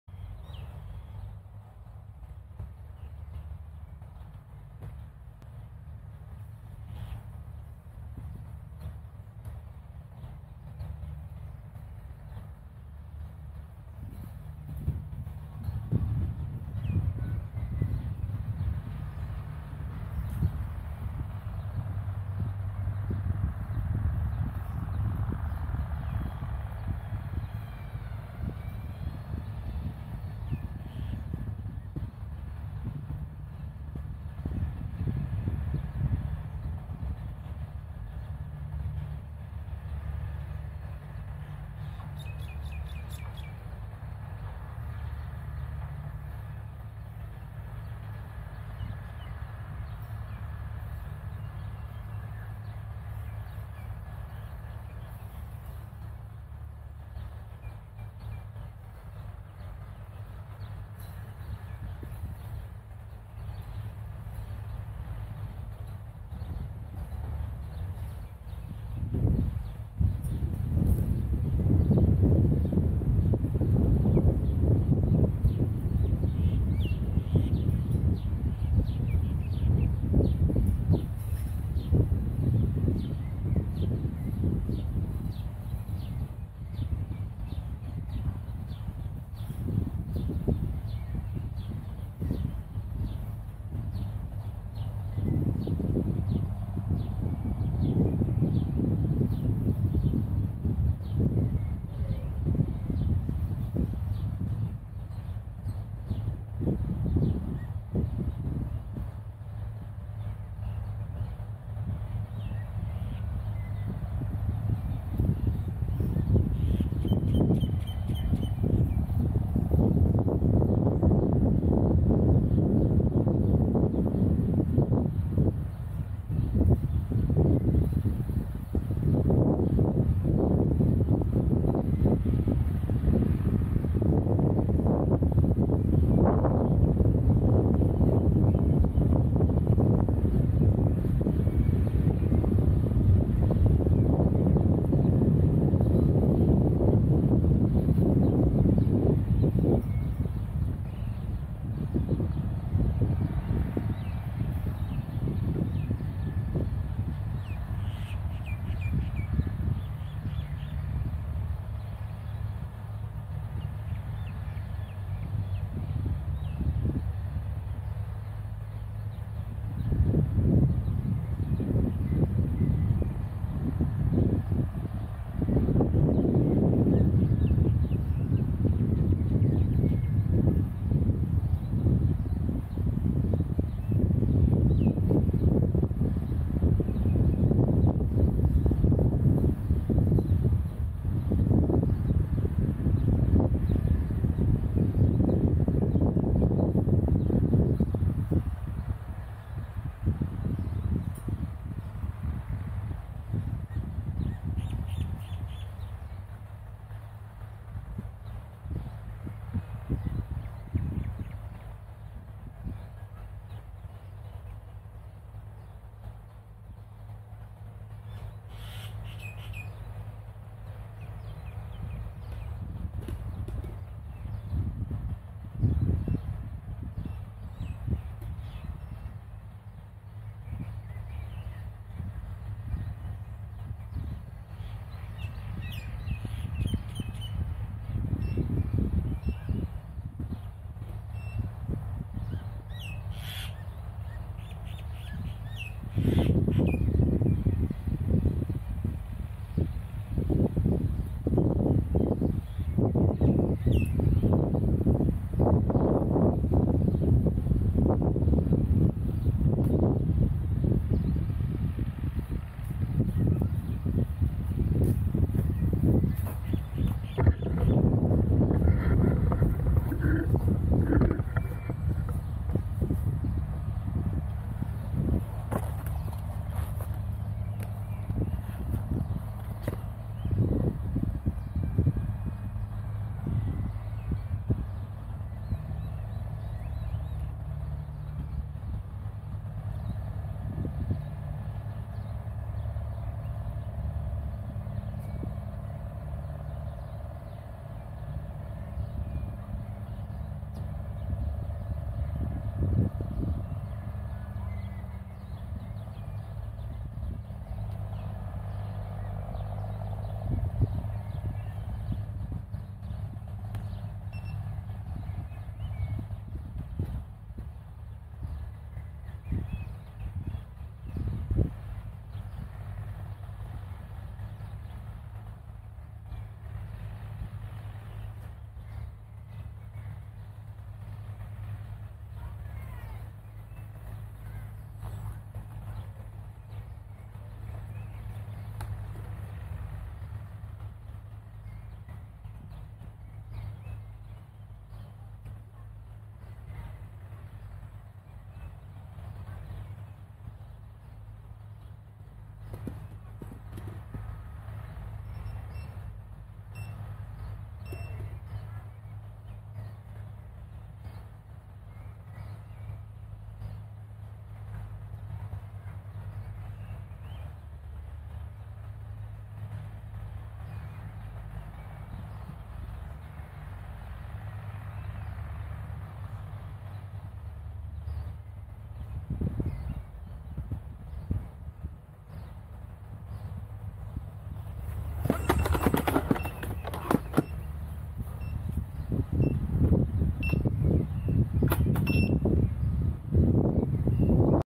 Riding an electric unicycle for the first time ever!
This is day 1. I spent around an hour or so learning to ride the wheel, starting by mounting holding a fence and rolling back & forth.  Once I was able to balance without the fence I stopped using it and just practised mounting and riding without the fence.  This is my first time ever on an electric unicycle and I was so hot, it was tough work.  I was on grass so I could just let it drop without fearing damage.